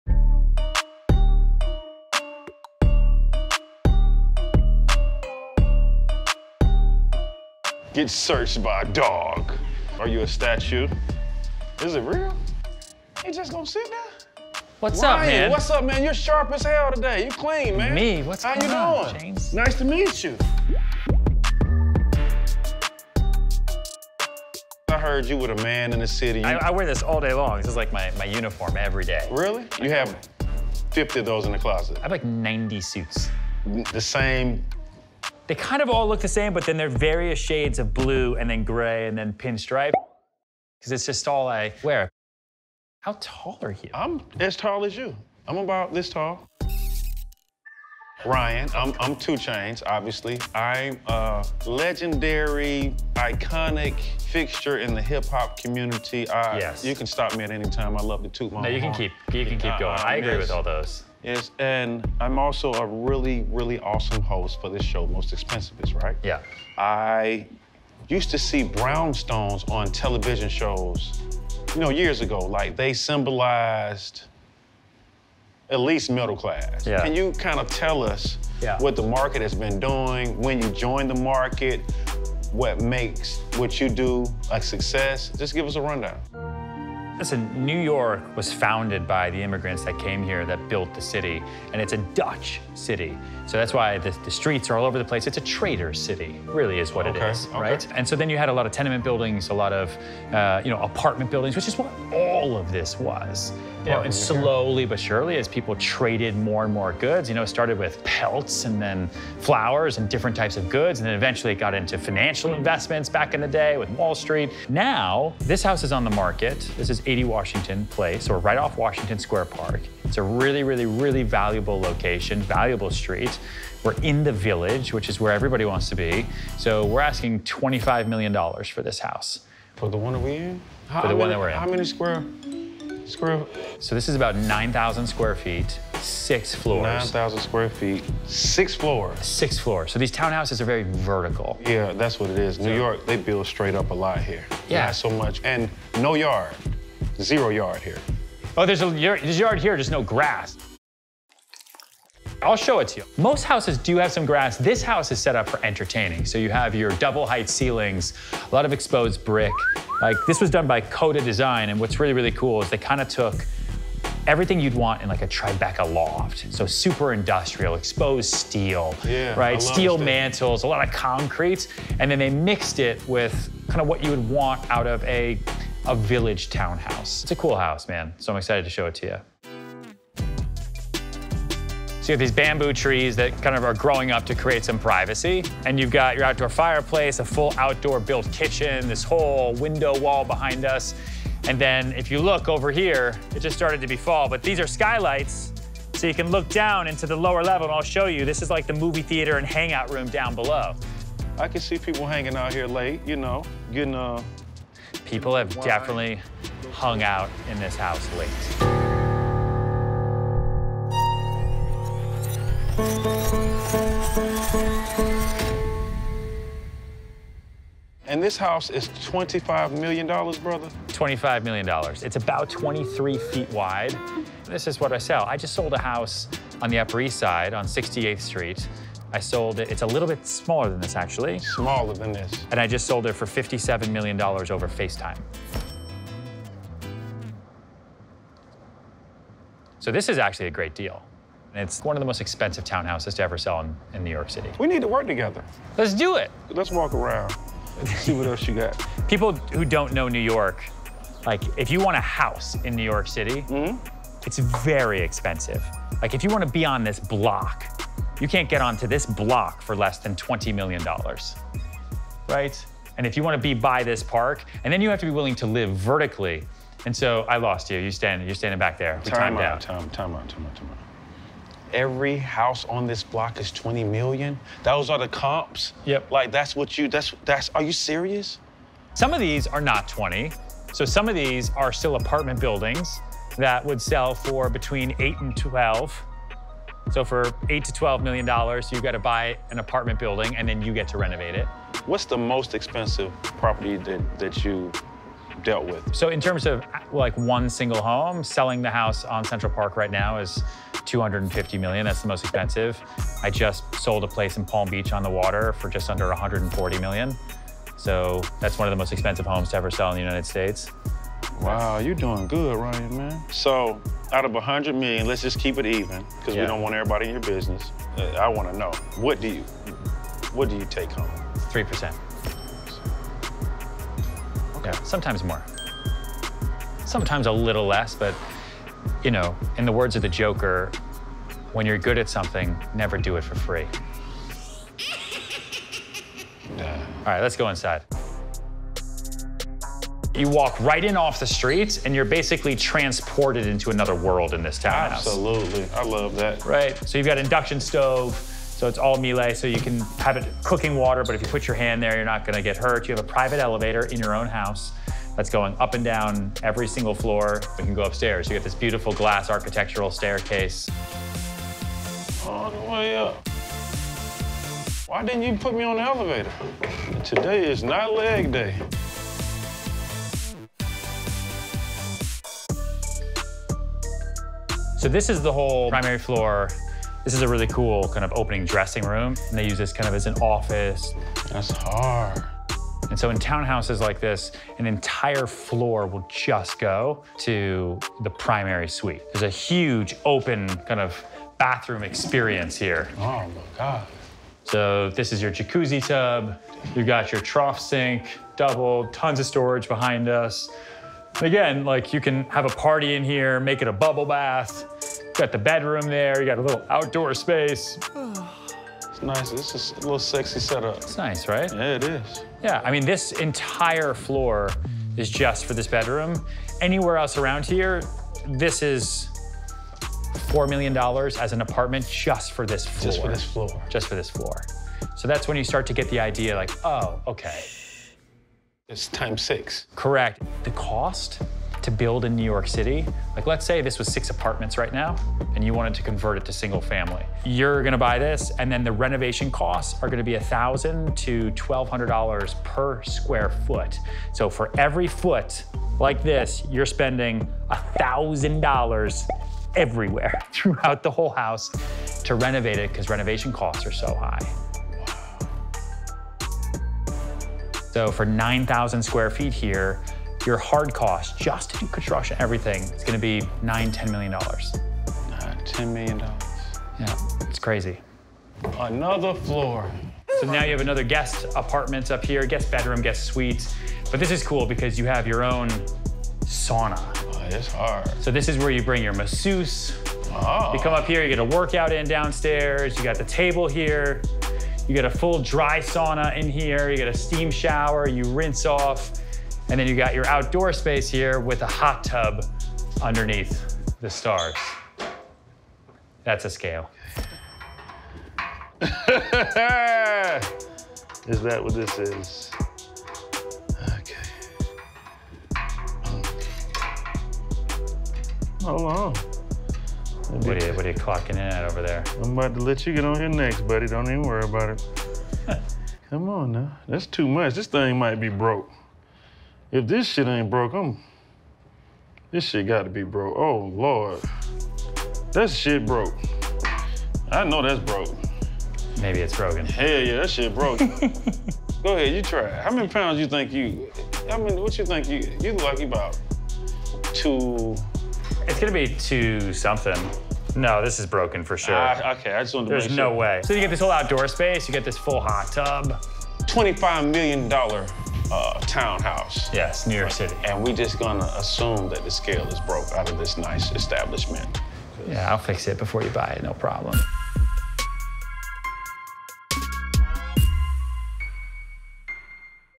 2 Chainz Goes Inside A $25 Million NYC Luxury Townhouse with Ryan Serhant | GQ
2 Chainz takes a look inside a $25 million dollar luxury townhouse with real estate broker and Million Dollar Listing star Ryan Serhant. This one-of-a-kind townhouse sits right on the border of Greenwich Village's Washington Square Park and is said to be the most expensive townhouse to ever sell in New York City.